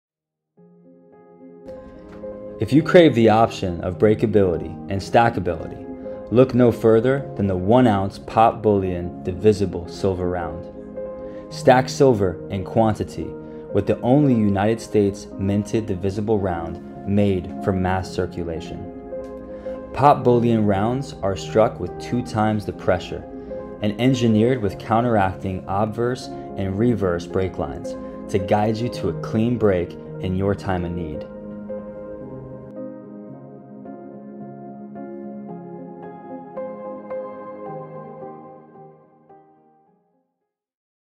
1 oz Pop Bullion Silver Round
Experience in the ultimate in convenience and stackability with the 1 oz Pop Bullion Silver Round. Each round can be divided into equal parts for easy breakability and trading in a time of need.